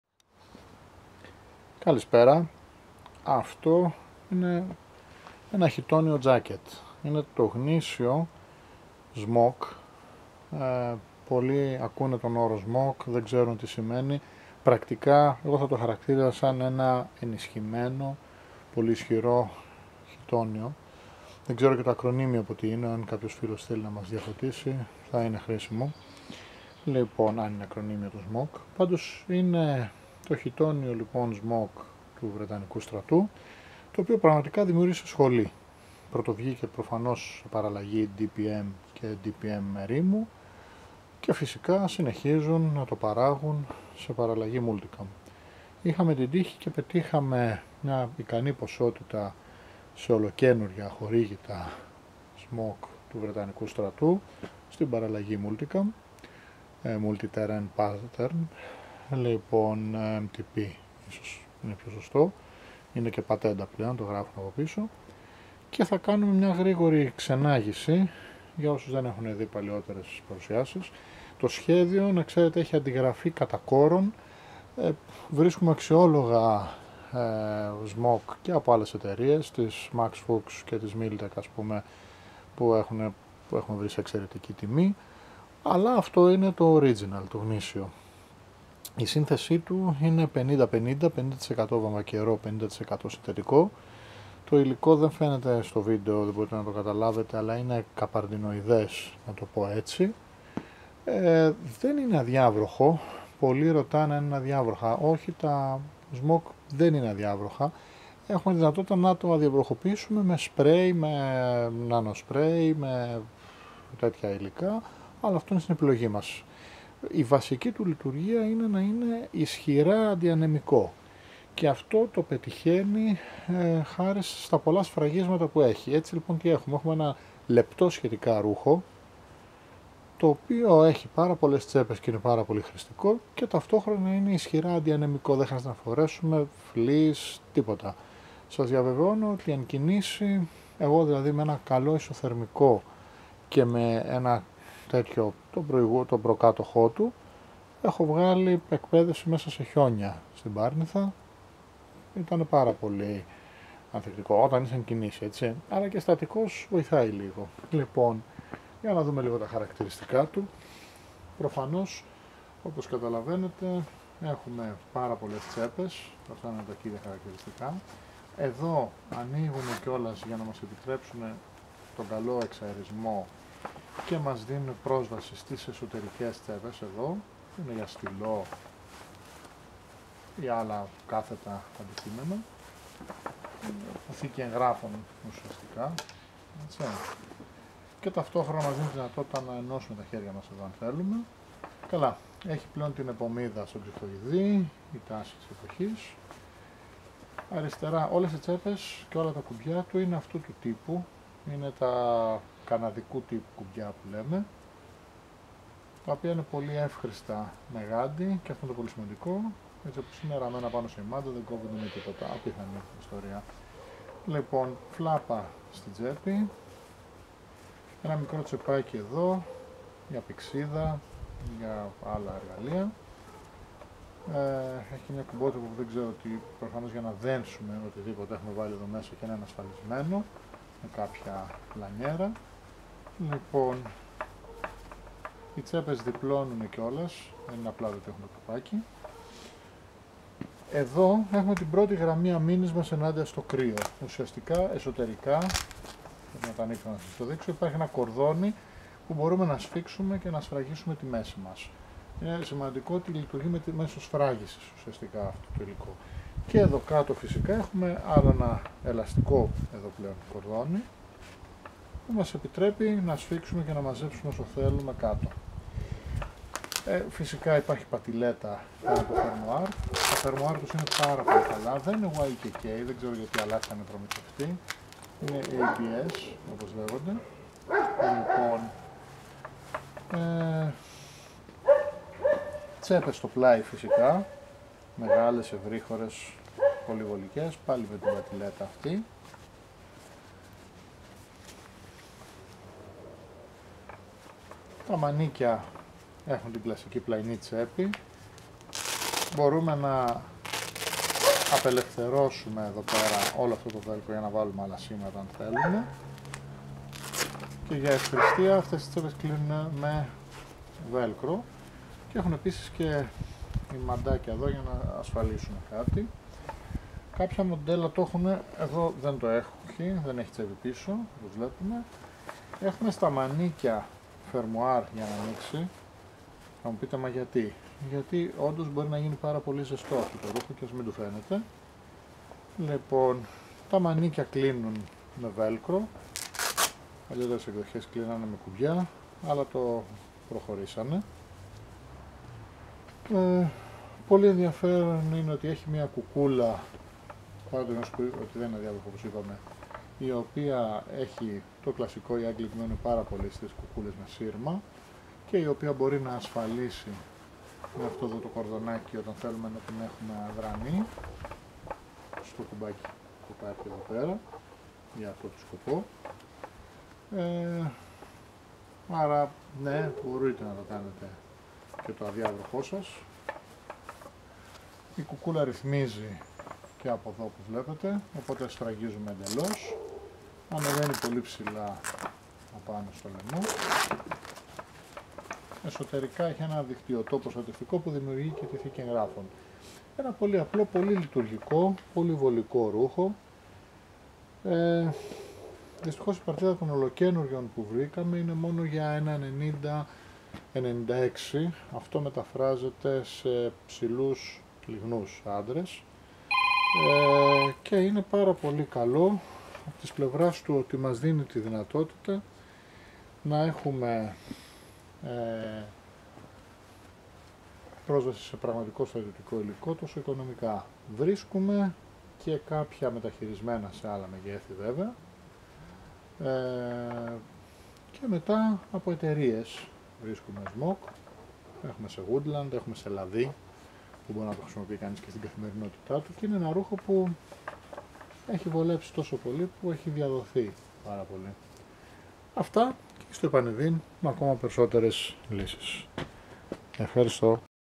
SMOCK MTP = 50€
-- Χορήγηση Βρεταννικού στρατού (αχορήγητα)
-- Διαθέσιμα μόνο σε 190/96 για ύψος 1,85-1,95 και περιφέρεια στήθους 92-100 εκ.
-- Σύνθεση: 50% βαμβάκι και 50% συνθετικό
-- Εξαιρετικά βολικό, ισχυρά αντιανεμικό, με μεγάλη χωρητικότητα ρούχο
-- 6 μεγάλες τσέπες:
2 στήθους
2 μέσης
2 ώμων
-- Velcro σήμανσης
-- 3 σημεία αμύνης στο κρύο:
μανσέτες που σφραγίζουν με velcro
κορδόνι μέσης
λάστιχο μηρών
-- Φερμουάρ στις μασχάλες
-- Κάθετα φερμουάρ πρόσβασης στο εσωτερικό, διχτυωτό για θήκη εγγράφων